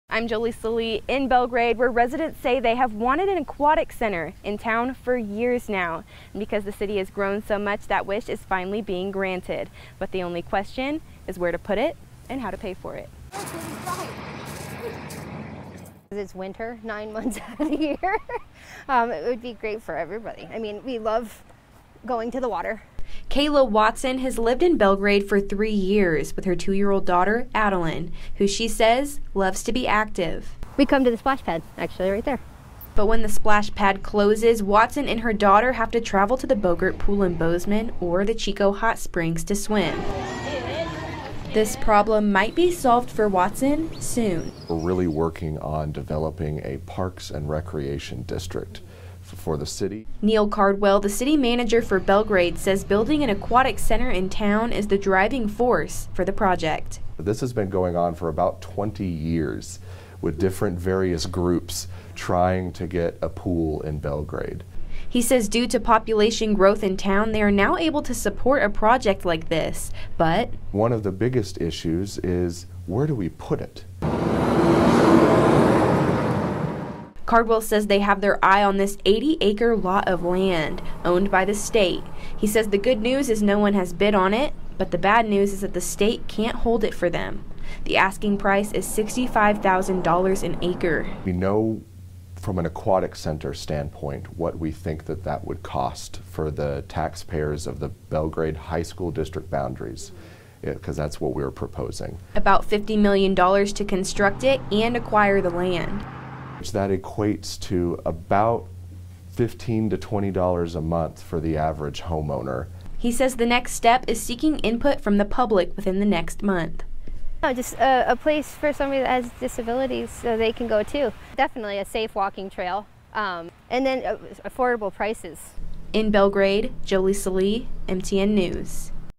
Belgrade one step closer to having a pool
Belgrade has its eye on a parcel of state-owned land for a public pool